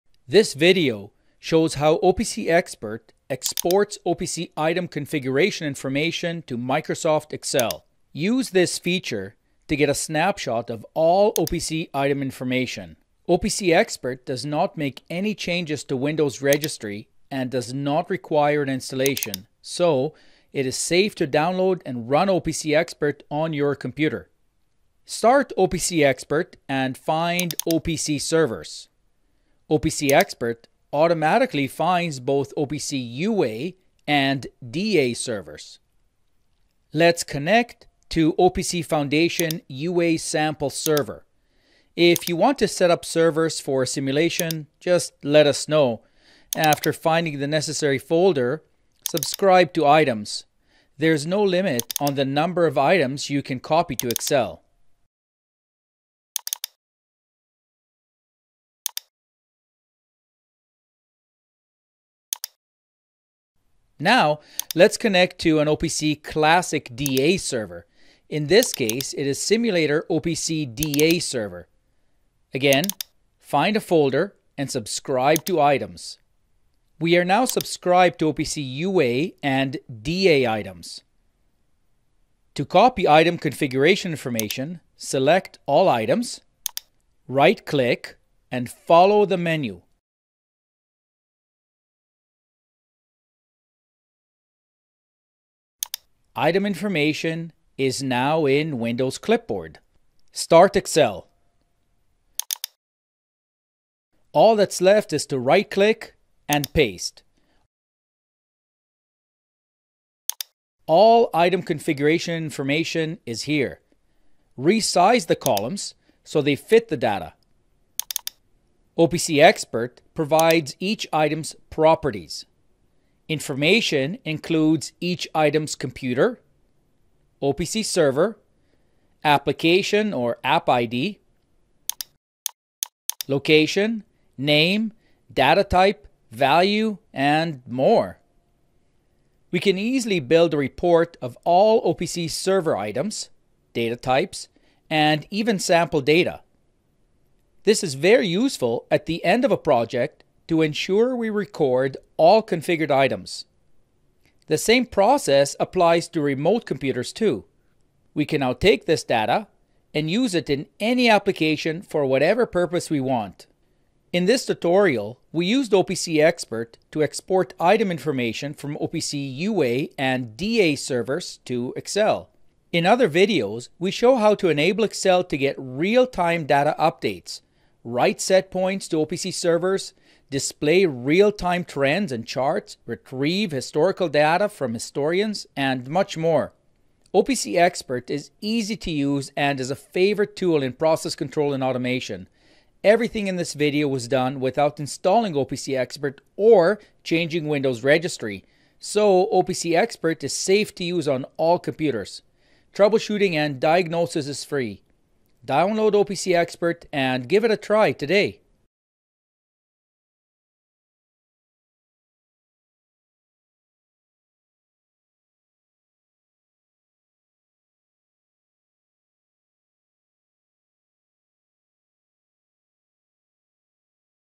OPC Expert - Export OPC Items to Excel
This video explains how to export OPC item configuration to OPC Excel, from both OPC UA and DA servers.